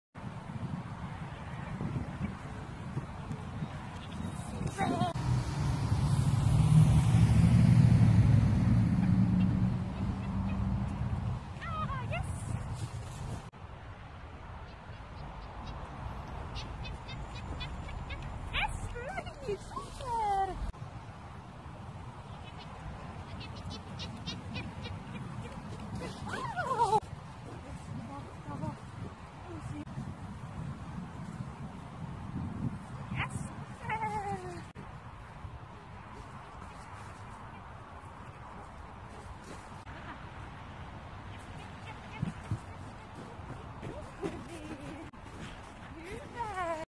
Gava keppitreeni vol 2, täydellistä! 16.3.2018
Keppitreenit toinen päivä, kaikki oikein!
- Vauhdilla kepeille
- vedätys/ohi juoksu
- valssi edessä tai päällejuoksu
- tiukka umpikulma